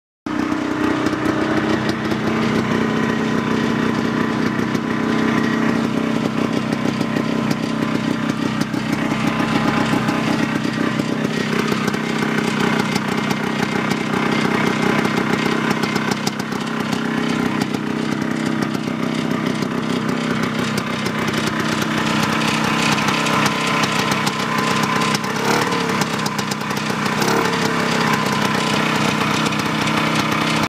1969 arctic cat panther P19J 297 JLO idle
1969 Arctic cat Panther P19J with 297 JLO L300 motor idle. just finished rebuilding the sled. runs great. just got done breaking trail through 30" of powder